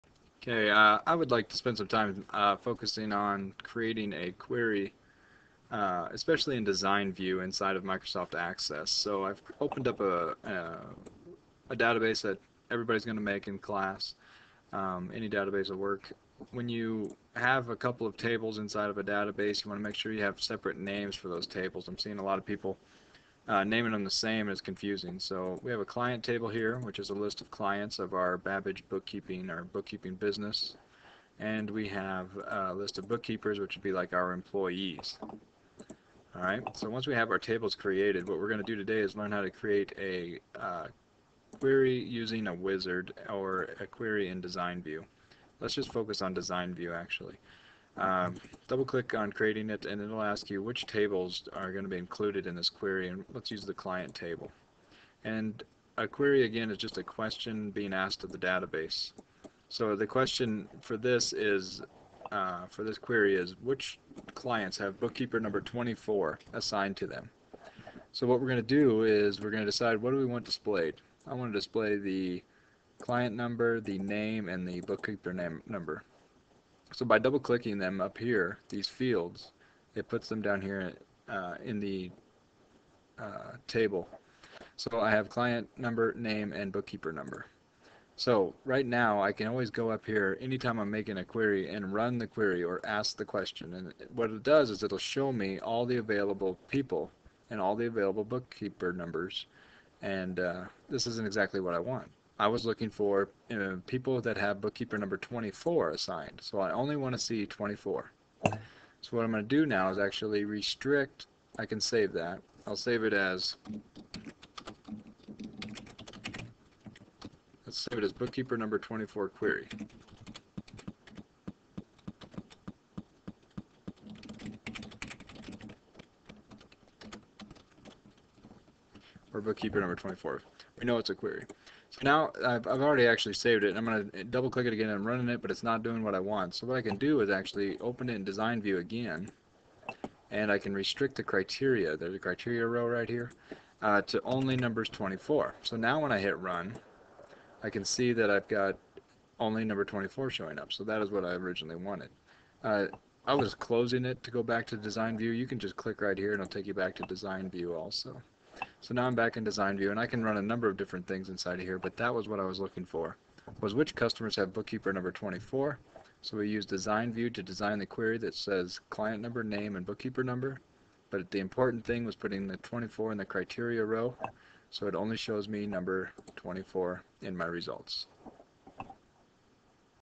Create Query in Design View
This video will display how to create a query in design view inside of Access 2003.